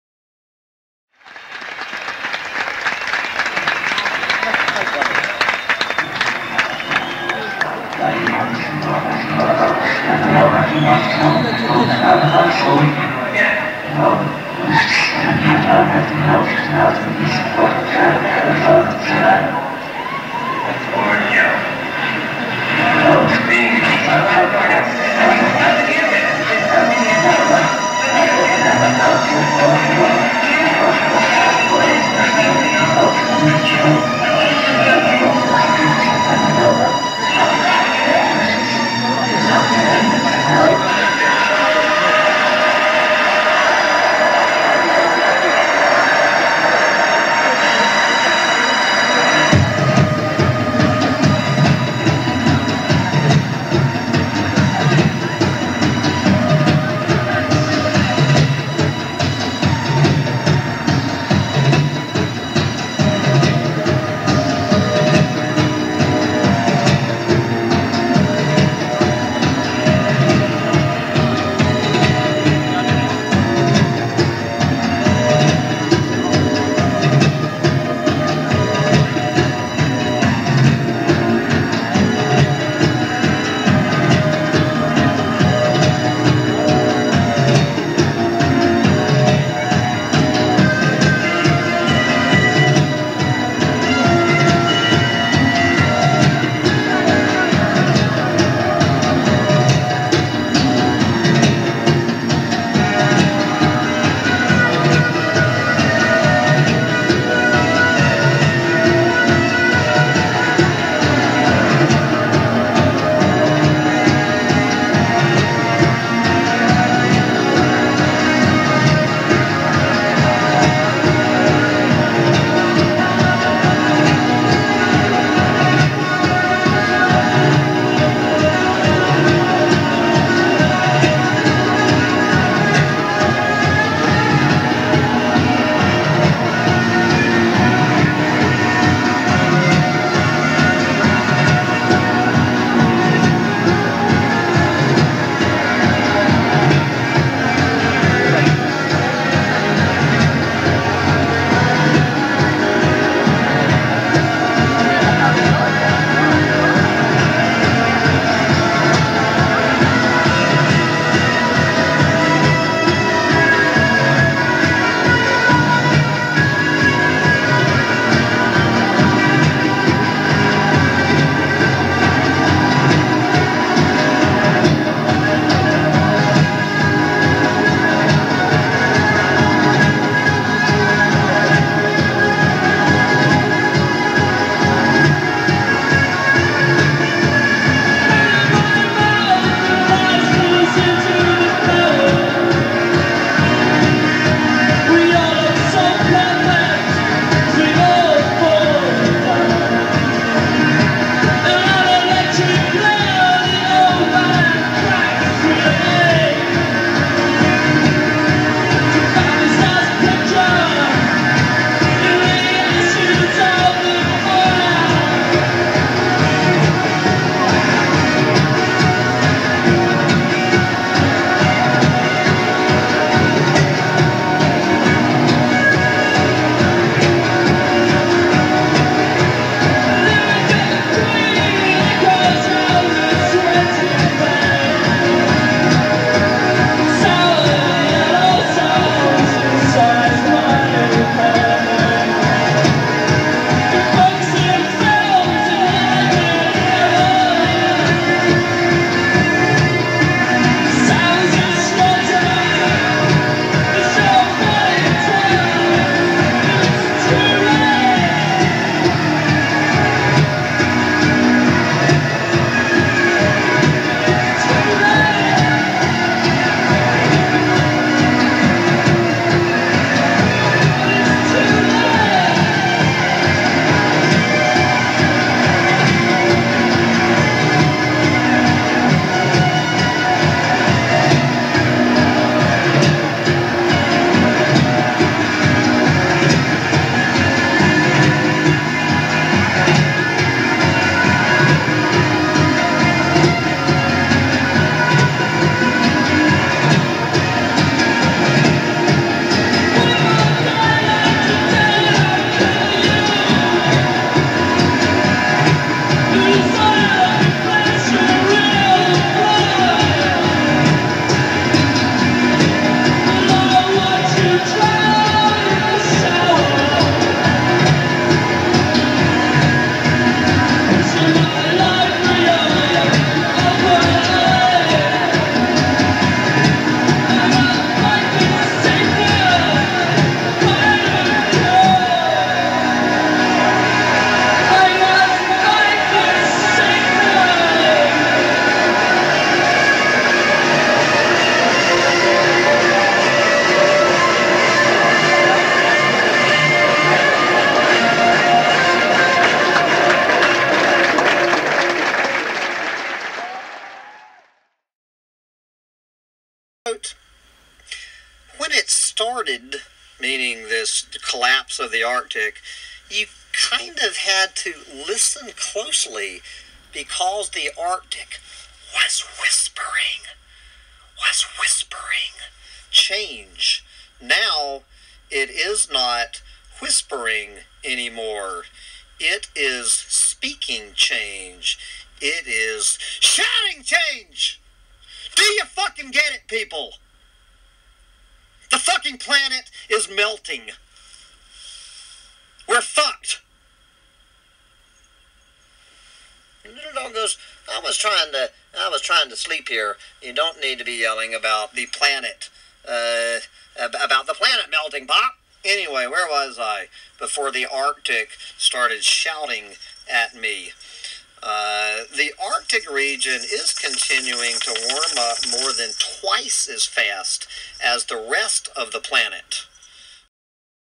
Arctic Sea Ice Collapse January 2017 — Part 3
Full collapse of Industrial Civilisation likely by early 2020s according to this analysis of Arctic sea ice extent and volume data from JAXA and the University of Washington 'PIOMAS' program.